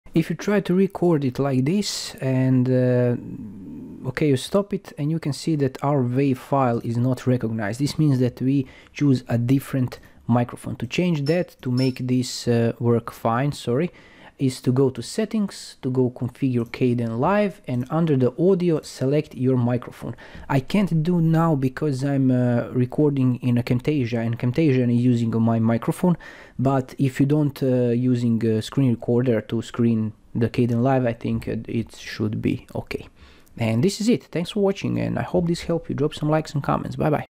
SOLVED! Microphone Doesn't Work - Cant Record Audio?
Learn how to select proper microphone for your audio narration recording process.